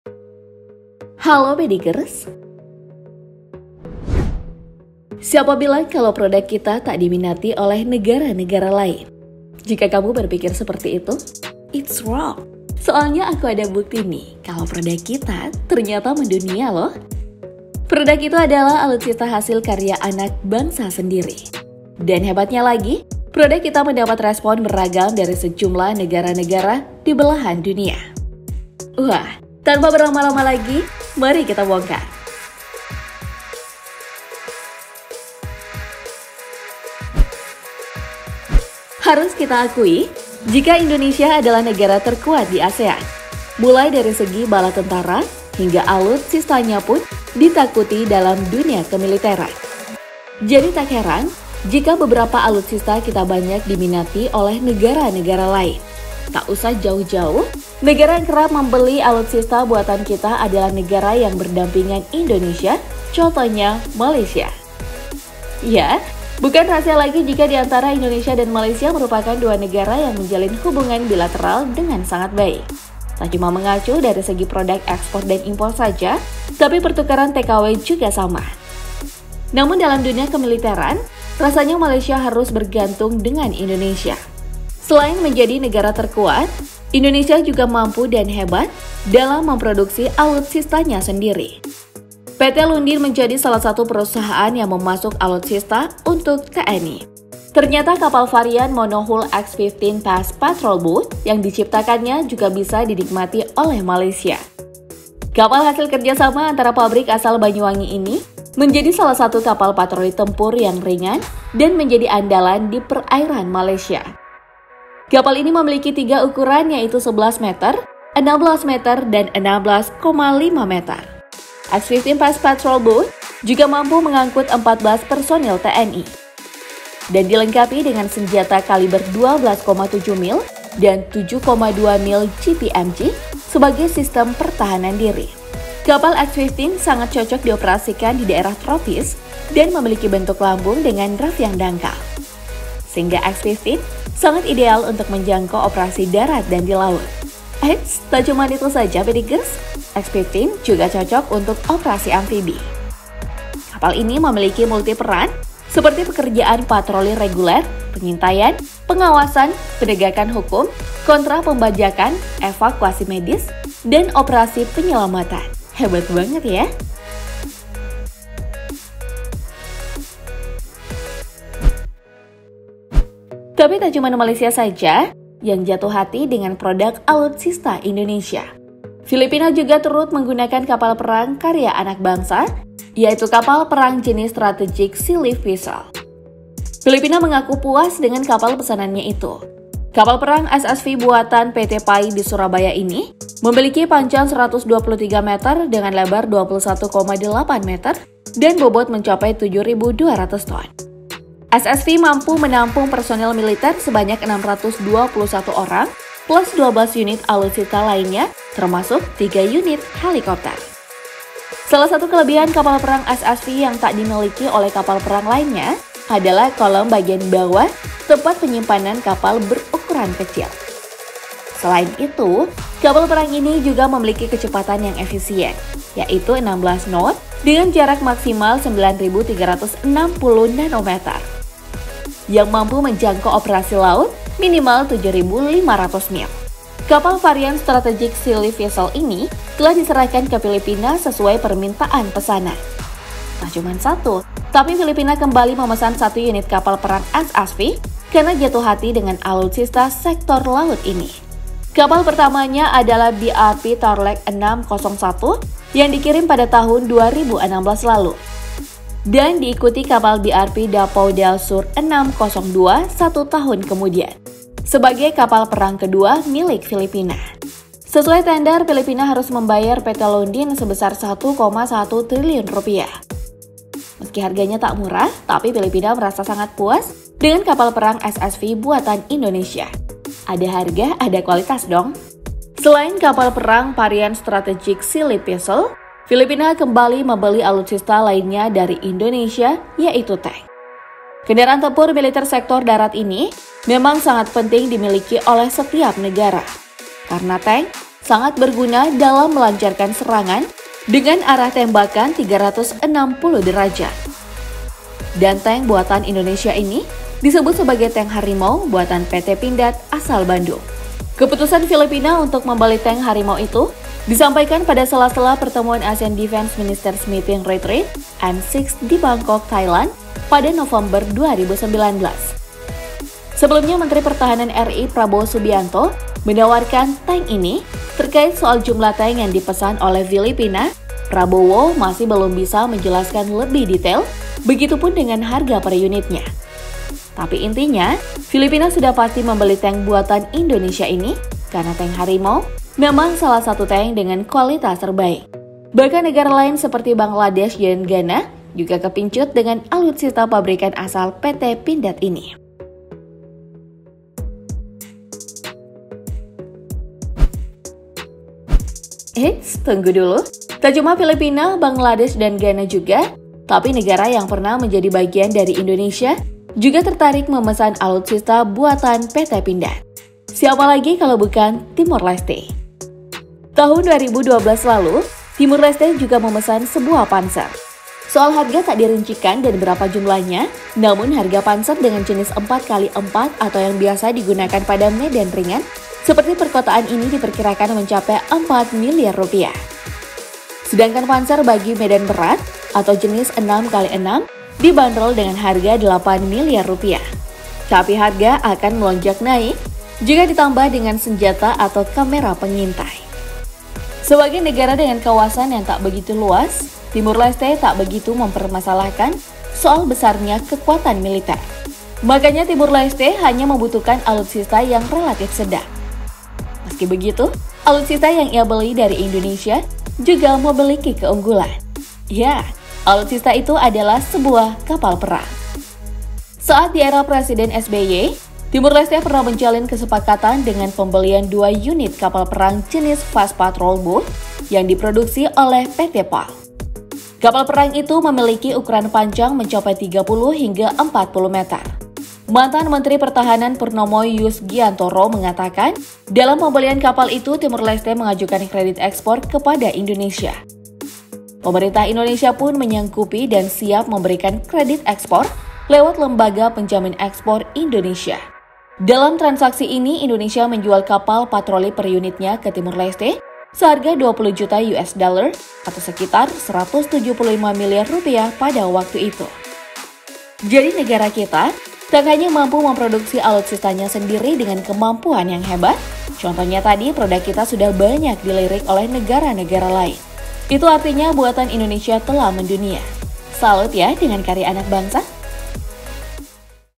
MENYESAL MEMBELI ALUTSISTA INDONESIA? Beginilah Respon Negara-Negara yg Beli Alutsista Buatan NKRI
Siapa bilang, kalau produk kita tak diminati oleh negara-negara lain? Jika kamu berpikir seperti itu, salah total yah. Soalnya aku ada bukti nih kalau produk kita mendunia loh. Produk itu adalah alutsista hasil karya anak bangsa sendiri. Dan hebatnya lagi nih, produk kita mendapat respon beragam dari sejumlah negara-negara di belahan dunia. Wah, tanpa berlama-lama lagi, yuk kita bongkar!